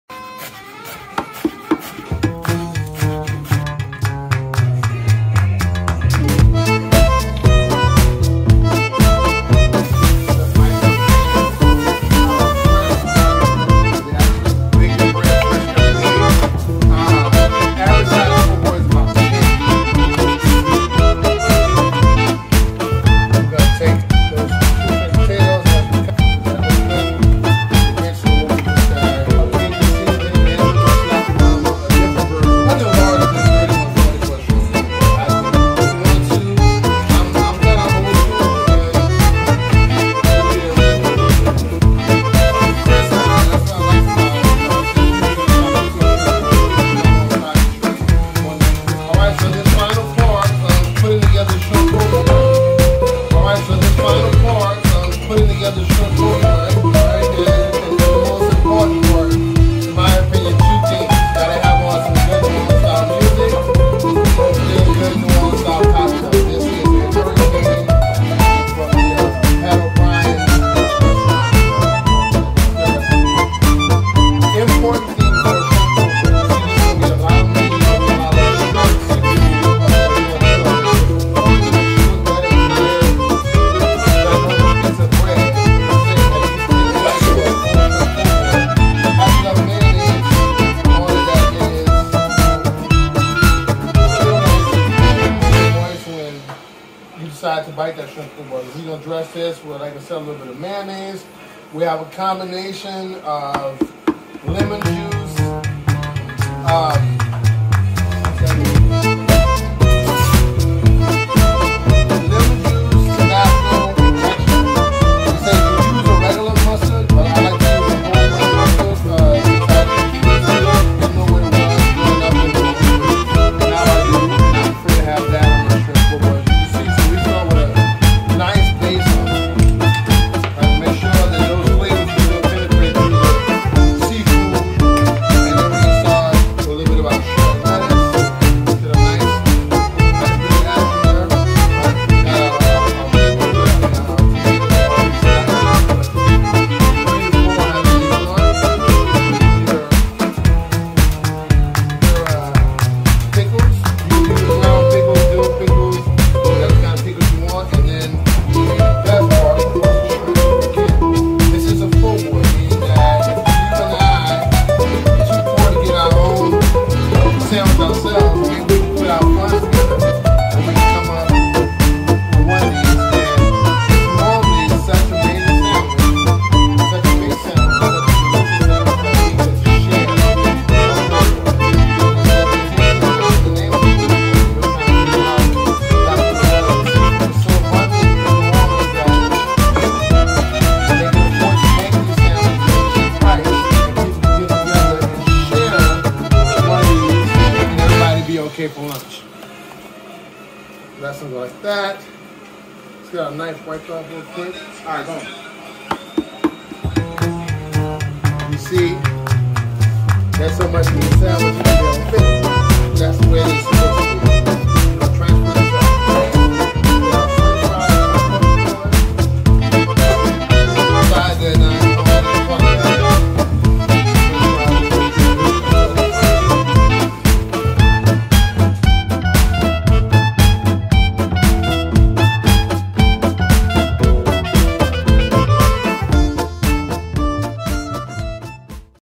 Faron's Shrimp Po'Boy Recipe
Our friend and chef Faron shares his classic New Orleans style shrimp o'boy recipe.  Laissez le bon temps rouler!